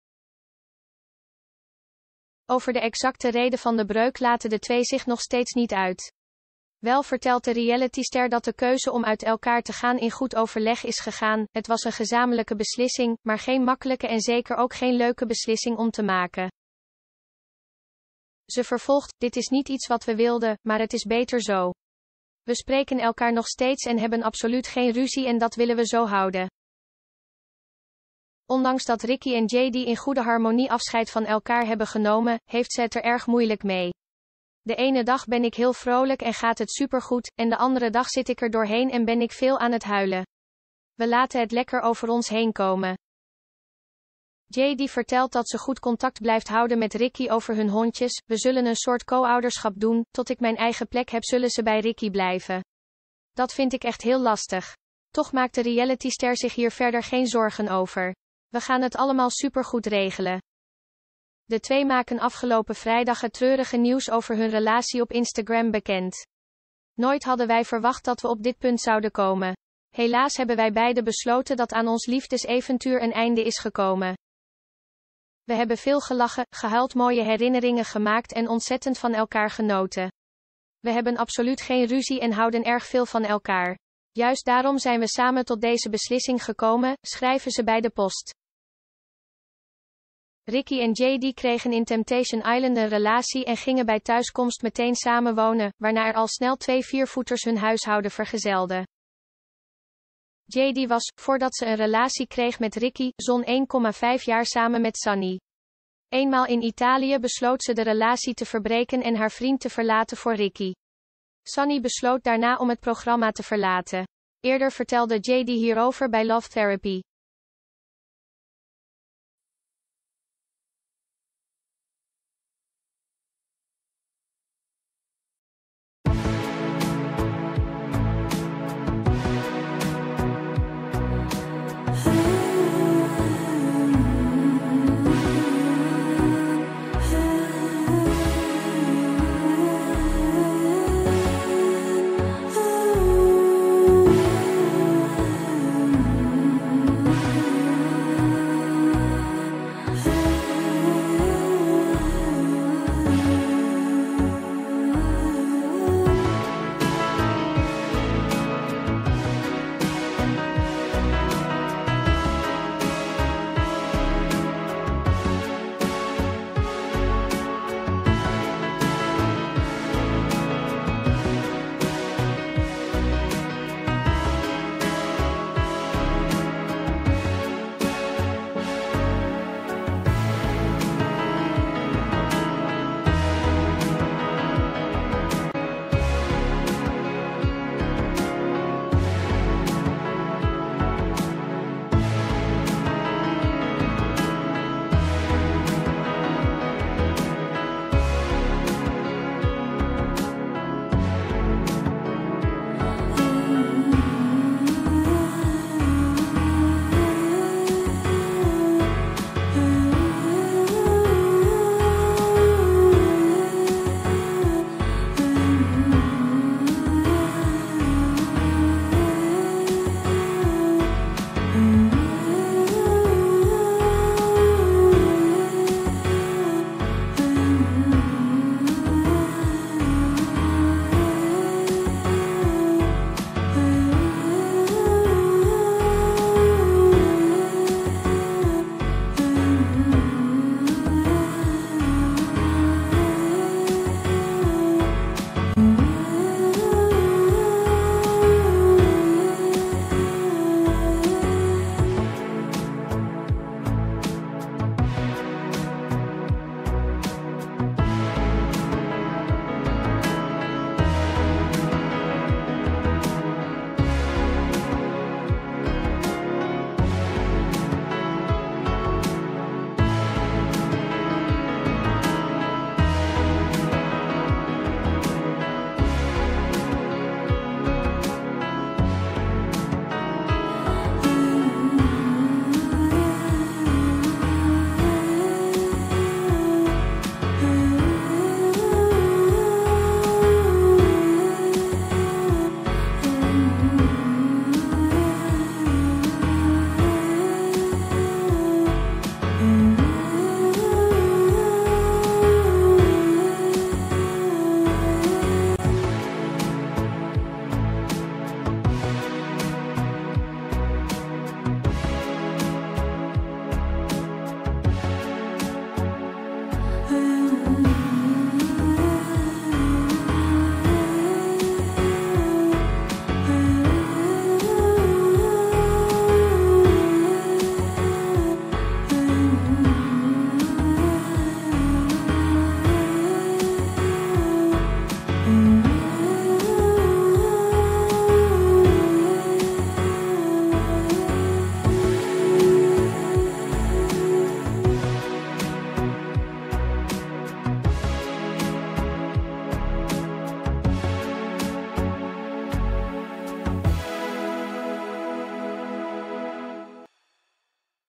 Jaydi uit Temptation over liefdesbreuk: 'Was geen makkelijke beslissing'
Afgelopen weekend maakten Ricky Lukassen (34) en Jaydi Dubois (23) - bekend uit 'Temptation Island' - het treurige nieuws wereldkundig een punt te zetten achter hun relatie. Veel mensen zagen deze breuk niet aankomen, waardoor het koppel overspoeld werd met vragen. Daarom besloot Jaydi duidelijkheid te geven op haar Instagram Stories via een 'Q&A'.